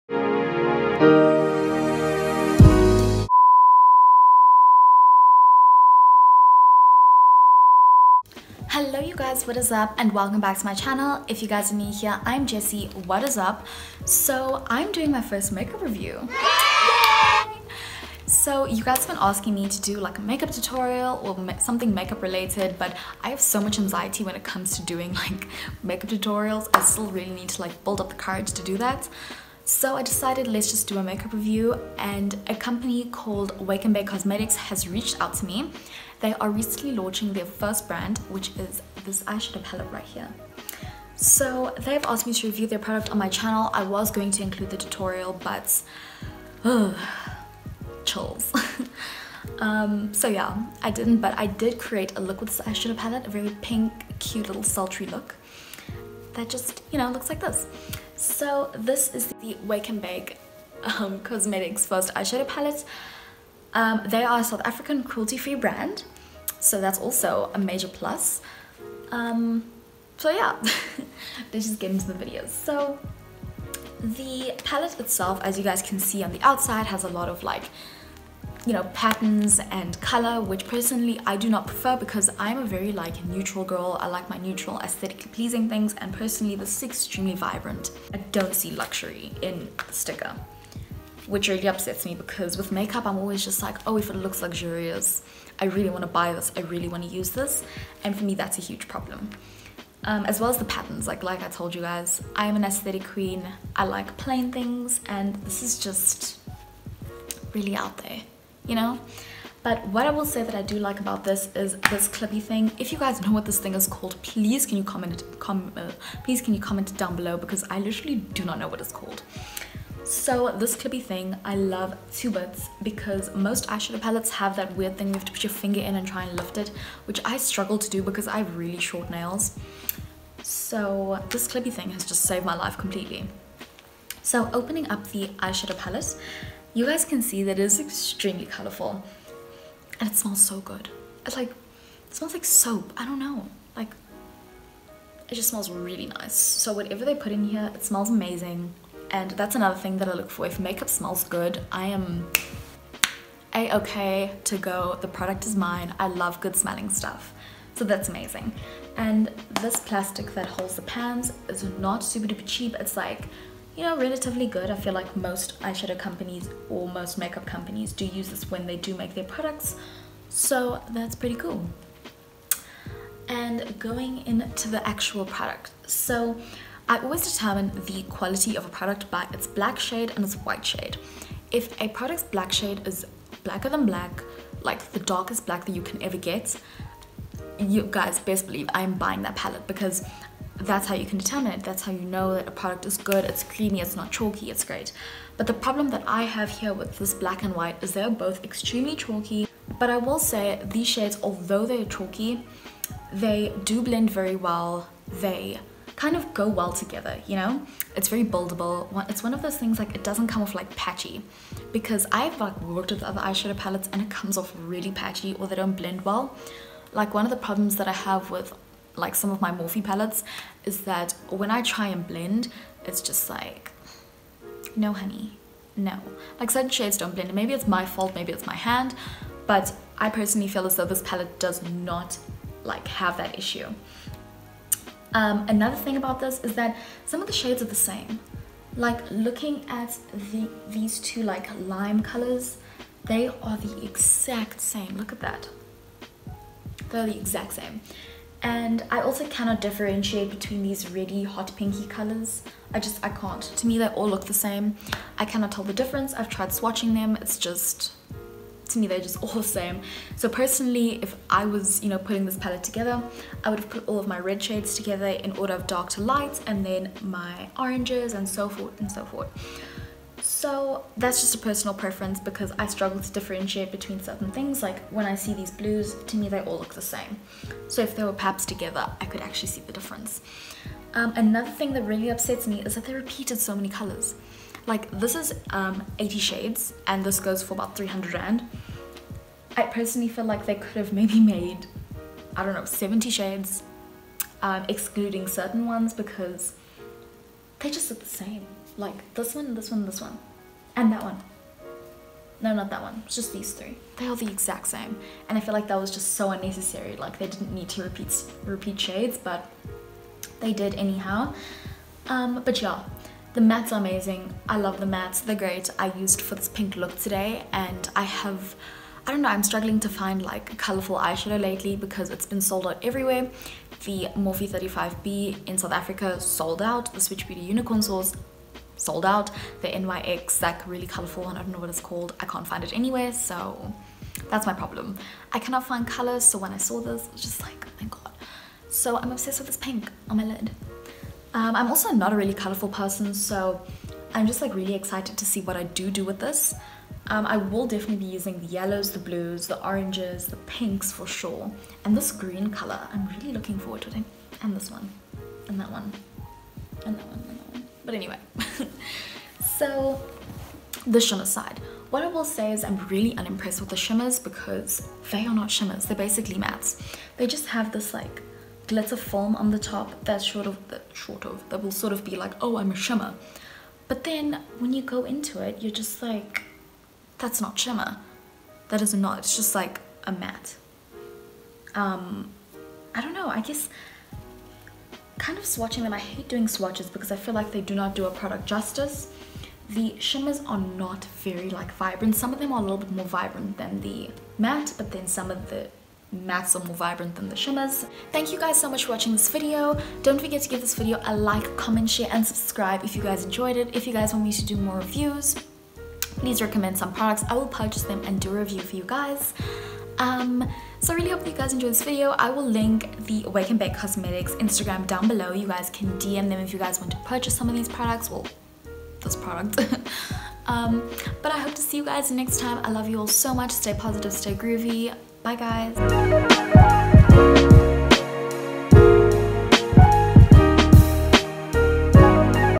BRUTALLY HONEST WAKE ‘N BAKE COSMETICS REVIEW
Hello my babies!
Today I’m coming at you guys with a make up review! Unfortunately I didn’t include a tutorial, but I will be posting one soon.
I hope that you all enjoy the video! I love you a million! Stay positive, stay groovy!

Ps, thank you for the support!